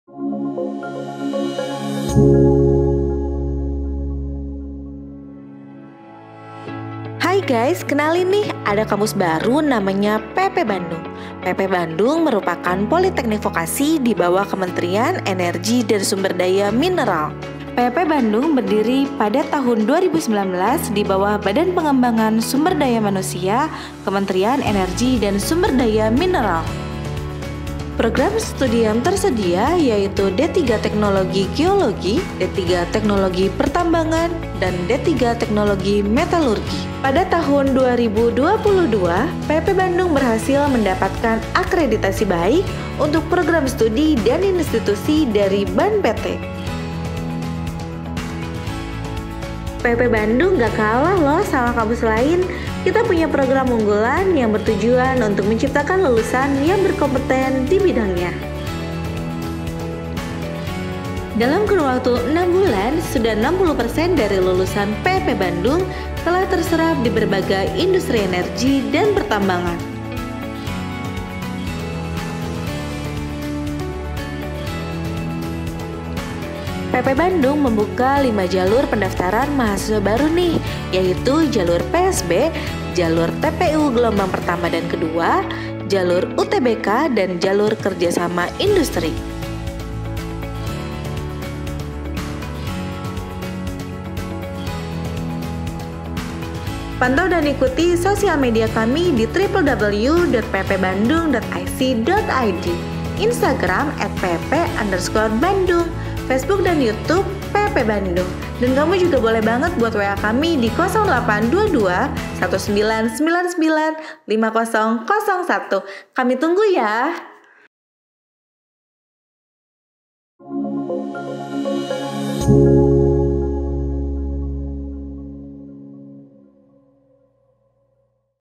PENERIMAAN MAHASISWA BARU POLITEKNIK ENERGI DAN PERTAMBANGAN BANDUNG Tahun Ajaran 2023/2024.
Politeknik Energi dan Pertambangan (PEP) Bandung adalah Politeknik dibawah Kementerian Energi dan Sumber Daya Mineral.

Kamu ingin berkuliah dengan sarana dan prasarana yang lengkap dan peralatan lab yang memadai? Mengasah skill keilmuan akademik di bidang pertambangan dan energi sekaligus skill organisasi? Ingin diajar tenaga profesional dan expert di bidang pertambangan dan energi? Atau ingin kuliah dengan banyak praktek? Jangan sia-siakan kesempatan berharga ini dan mari bergabung bersama kami.🤩
PEP Bandung membuka pendaftaran Mahasiswa Baru Tahun Ajaran 2023/2024 dengan pilihan Program Studi :
1. D3 Teknologi Geologi
2. D3 Teknologi Pertambangan
3. D3 Teknologi Metalurgi

Terdiri dari 4 (empat) jalur
1. Jalur PSB (Penelusuran Siswa Berprestasi)
2. Jalur UTBK (Ujian Tulis Berbasis Komputer)
3. Jalur TPIU (Tes Potensi Intelektual Umum)
4. Jalur kerjasama

⏱️Timeline Pendaftaran⏱️
📆 Jalur PSB : 9 Januari - 3 Februari 2023
📆 Jalur TPIU Gelombang 1 : 20 - 31 Maret 2023
📆 Jalur TPIU Gelombang 2 : 29 Mei - 9 Juni 2023
📆 Jalur UTBK : 21 Juni - 11 Juli 2023